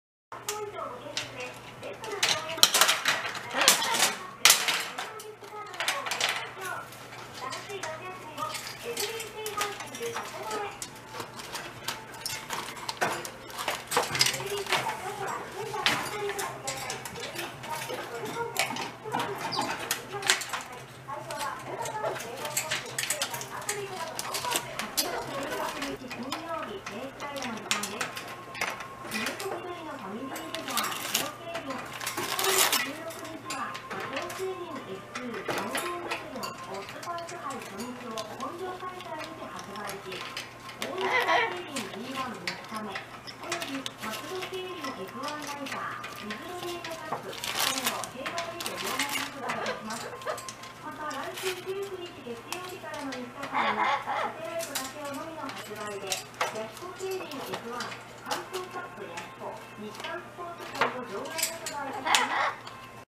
宝物隠しをするタイハクオウム
ケージの床に敷いてる新聞紙を、引っ張り出しては小さく切って
その下に「寅」の宝物を入れようとしています。
見てると隠したり、また出したりと忙しいことです。